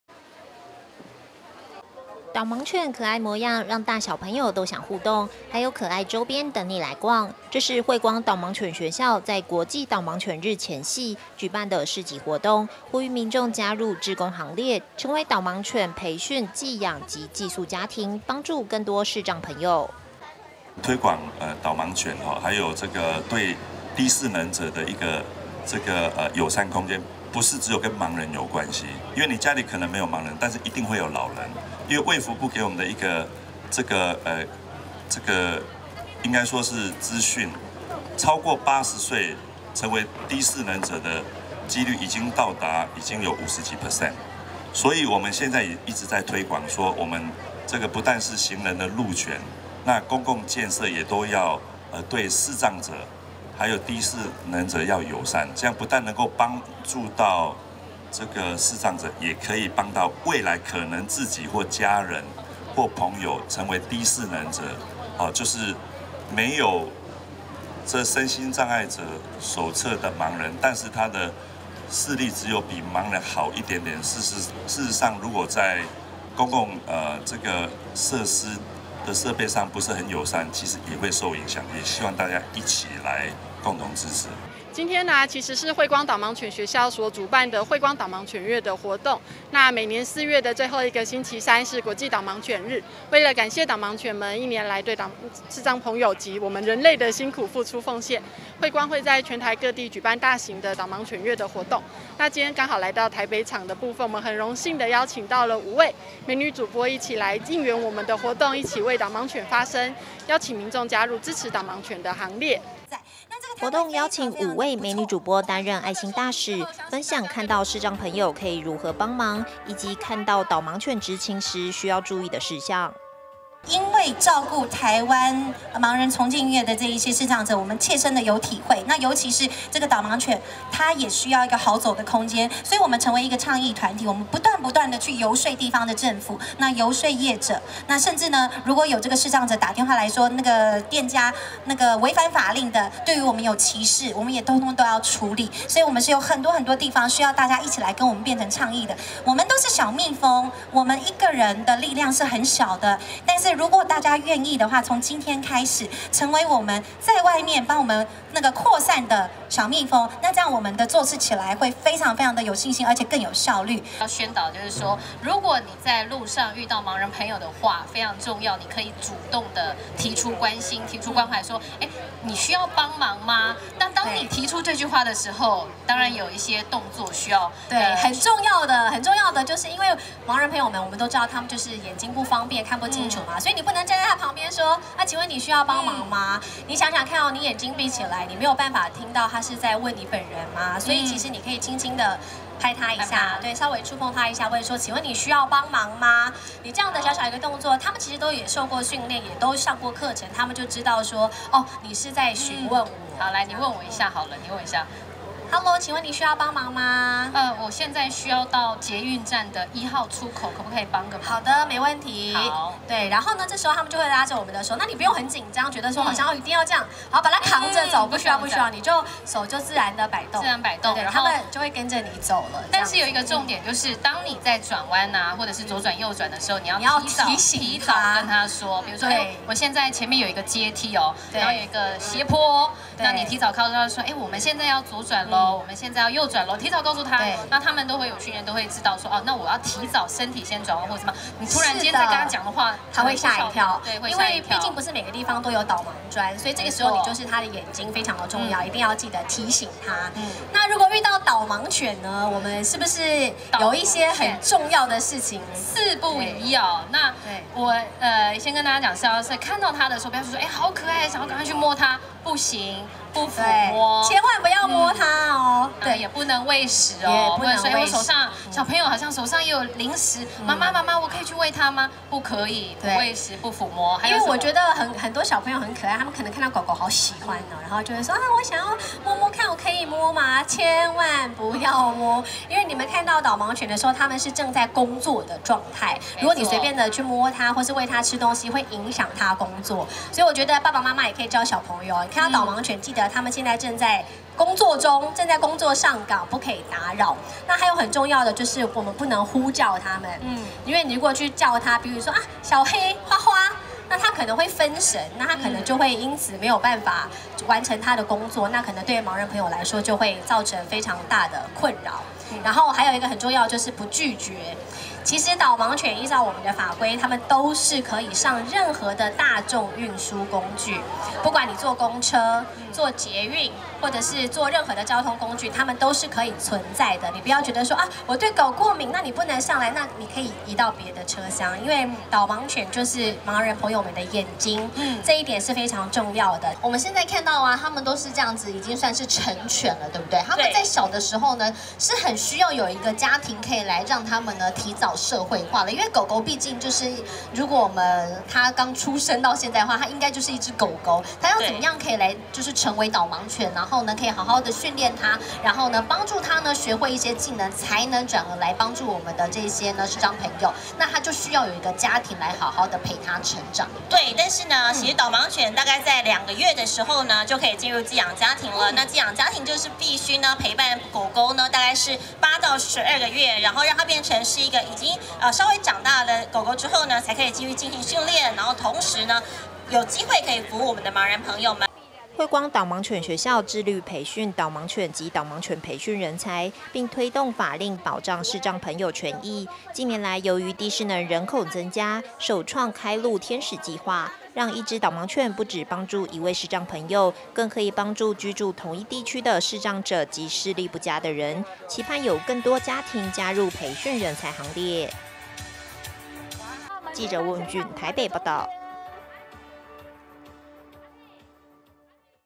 拉布拉多|導盲犬|國際導盲犬日 盼民眾加入志工行列
國際導盲犬日 盼民眾加入志工行列

國際導盲犬日將近，惠光導盲犬學校邀請5位美女主播推廣導盲犬正確觀念，也盼民眾加入培訓狗狗，成為導盲犬寄養家庭，幫助視障及低視能朋友，或是視力不佳的長輩。

導盲犬可愛模樣，讓大小朋友都想互動，還有可愛周邊等你來逛，這是惠光導盲犬學校在國際導盲犬日前夕，舉辦的市集活動，呼籲民眾加入志工行列，成為導盲犬寄養及寄宿家庭，幫助更多視障朋友。

活動邀請５位美女主播擔任愛心大使，分享看到視障朋友可以如何幫忙，以及看到導盲犬執勤時需要注意的事項。

惠光導盲犬學校致力培訓導盲犬及導盲犬培訓人才，並推動法令保障視障朋友權益，近年來由於低視能人口增加，首創開路天使計劃，讓一隻導盲犬不只幫助一位視障朋友，更可以幫助居住同一地區的視障者與視力不佳的人，期盼有更多家庭加入培訓人才行列。